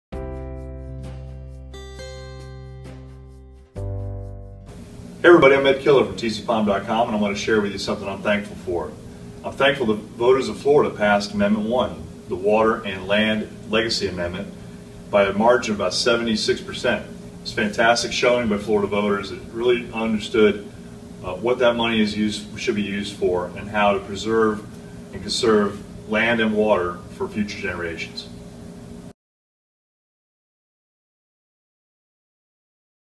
Season of Thanks: Ed Killer
Outdoors expert and Indian River Lagoon team member Ed Killer shares something he is thankful for.